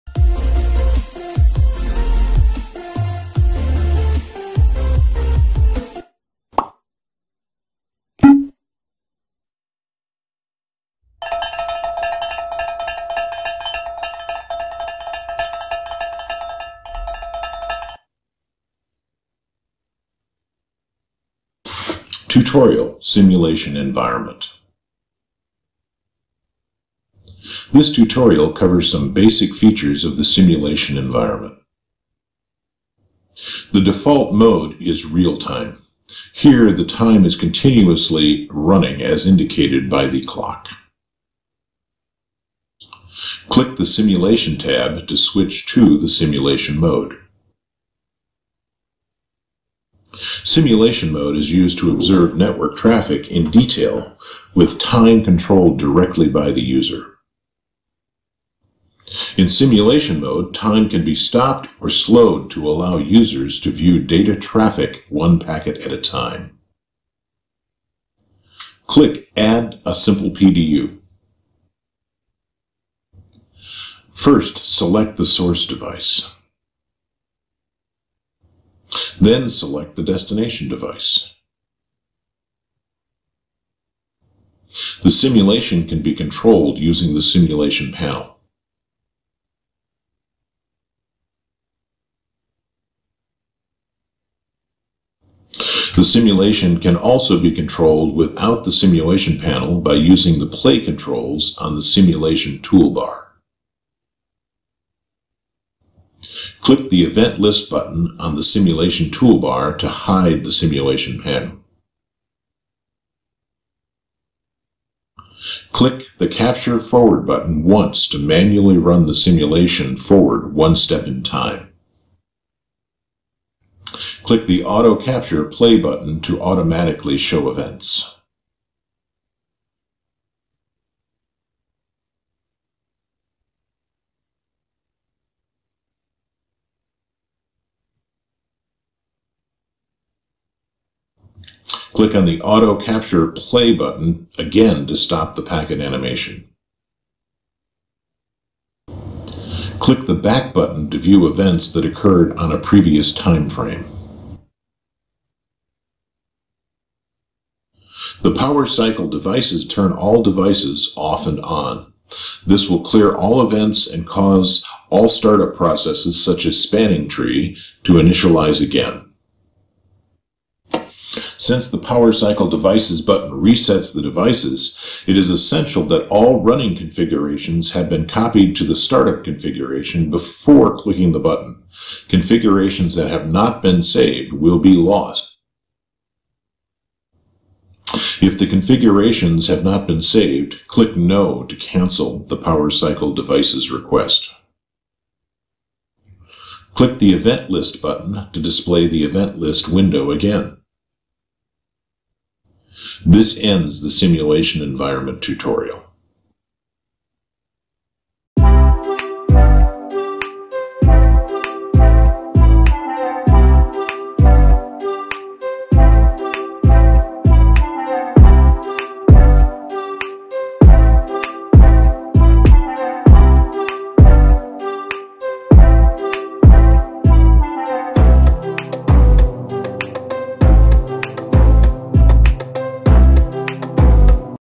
Simulation Environment | Official Packet Tracer Tutorials | 4 Realtime and Simulation Modes
Disclamer: This Video Content Has been made available for informational and educational purposes only

Official Packet Tracer Tutorials

The following tutorials demonstrate the basic functions, features, and aspects of Packet Tracer.

4 Realtime and Simulation Modes

4-1 Simulation Environment

- Introduces an overview of the simulation environment.